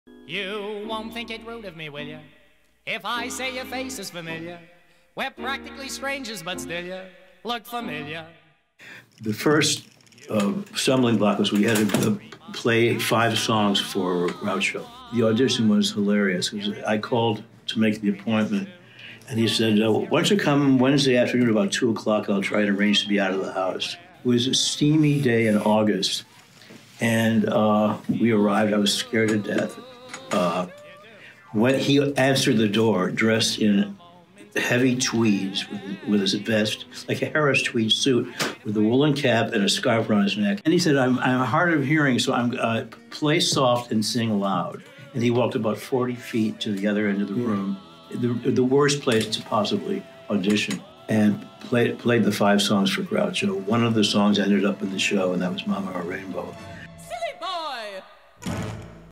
Minnie's Boys Auditioning for Groucho
Before they could get the rights to do Minnie's Boys, the show's creators had to audition for Groucho Marx.

One night only, Sunday February 10, 2019 at 7PM
Alex Theatre, Glendale